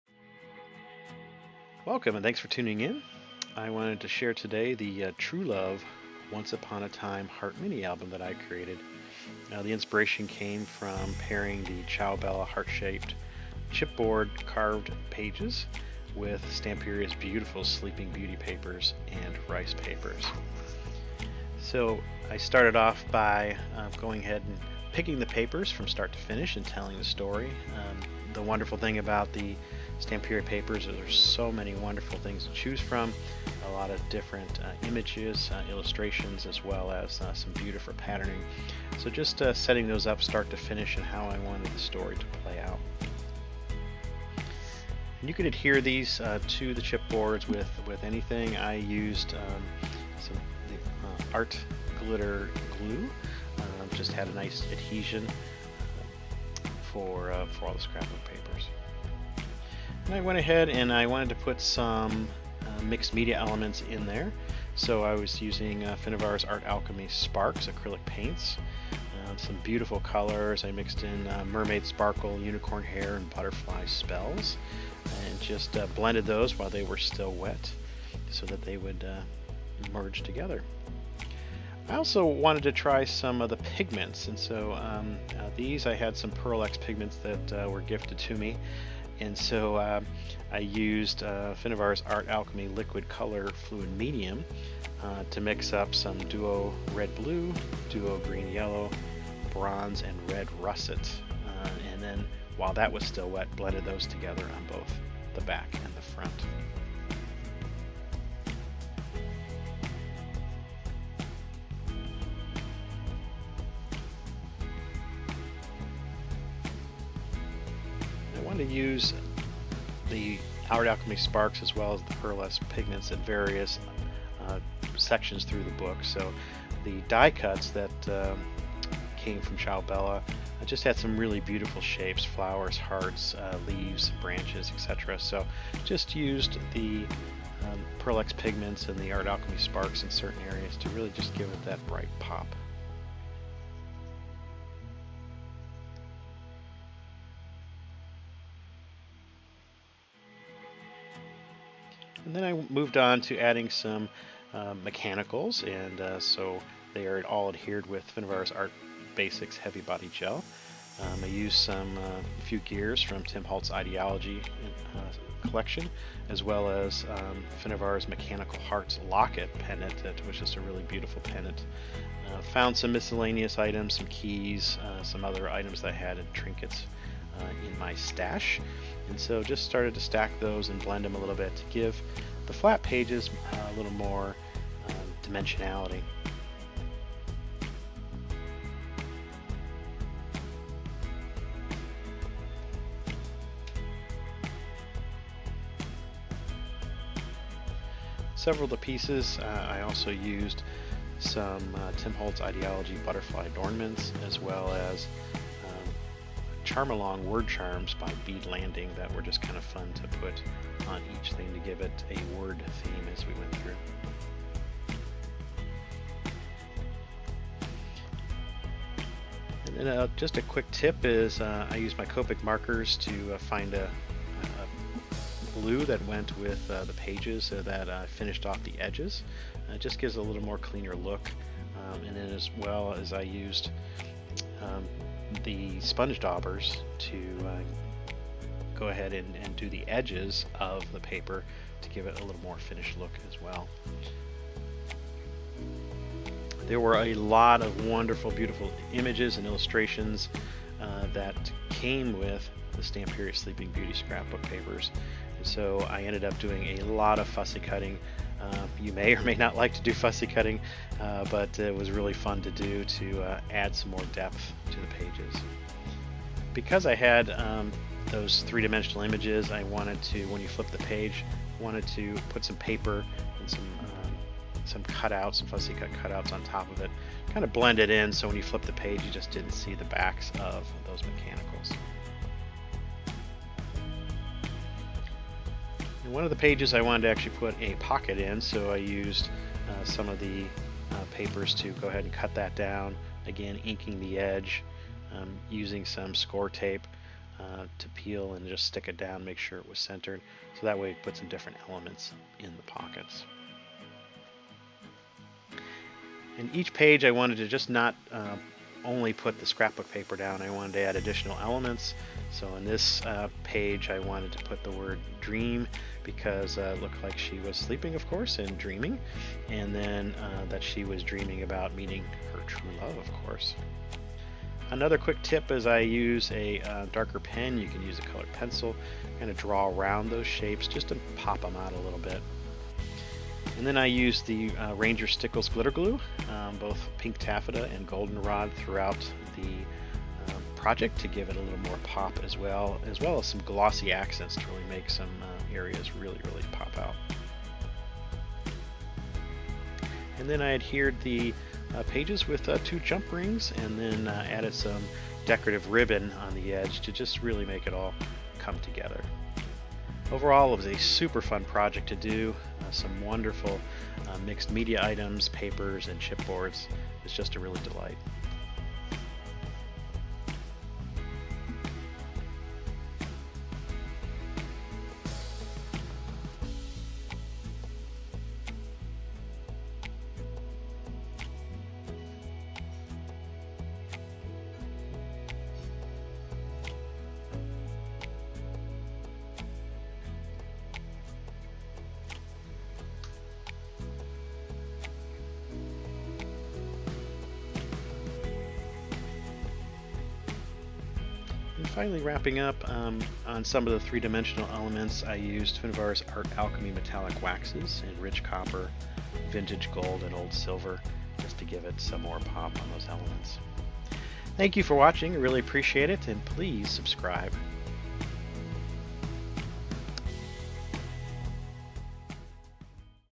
True Love Once Upon a Time Heart Mini Album
Inspiration: I paired the Cia Bella Heart Shaped Chip Board Carved Pages with Stamperia’s beautiful Sleeping Beauty papers and rice papers and added mixed media and mechanical elements to create this unique 28-page True Love Once Upon a Time Heart Mini-Album.

Description: The True Love Once Upon a Time Heart Mini-Album features 4 carved heart pages with various designs, 6 thick heart pages and 4 thin heart pages bound with jump rings. The piece measures 10” tall x 8.5” wide and 2.5” deep with loads of space for photos and memorabilia on 20 pages.

On the decorative pages, I used mixed media techniques for the magical colors accented with mechanical elements. The album is decorated throughout using the beautiful Sleeping Beauty papers and rice papers by Stamperia. The album is filled with fussy-cut images, Stickles glitter areas and Glossy Accents to give elements a three-dimensional look. I finished off the album with a pair of jump rings and various colorful ribbons.

Products:
- Cia Bella Heart Shaped Designer Chip Board Carved Pages and Solid Heart Pages
- Stamperia Sleeping Beauty scrapbook papers
- Stamperia Sleeping Beauty decoupage rice papers
- Art Glitter Glue
- Versa Magic Ink - Eggplant
- Color Box Pigment Brush Pad – Gold Rush
- Art Alchemy Sparks acrylic paints – Mermaid Sparkle, Unicorn Hair, and - - Butterfly Spells
- Pearl Ex Pigments – Duo Red-Blue, Duo Green-Yellow, Bronze, and Red-Russet
- Art Alchemy Liquid Color Fluid Medium
- Finnabair Art Basics Heavy Body Gel
- Tim Holtz idea-ology Industrial Metal Gears
- Prima Marketing Finnabair Mechanicals Heart Locket Pendants
- Prima Marketing Finnabair Mechanicals Tin Hearts Metal Embellishments
- Tim Holtz idea-ology Butterfly Adornments
- Bead Landing Charmalong Word Charms
- Finnabair Art Alchemy Metallique Wax – Rich Copper, Vintage Gold, and Old Silver
- Ranger Stickles Glitter Glue - Golden Rod
- Ranger Stickles Glitter Glue - Pink Taffeta
- Glossy Accents
- Jump Rings
- Ribbon